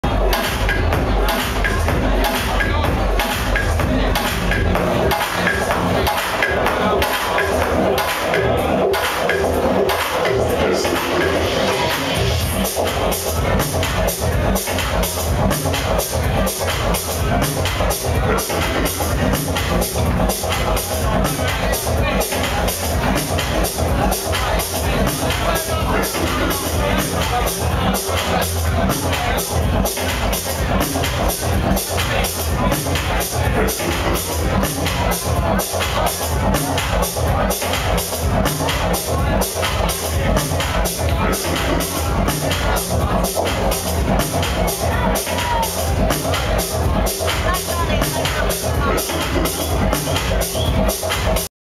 Jaded at Cable 1 year ft konrad Black 06.03.11
The new Jaded visuals appear for the 1st time in Cable's bar room.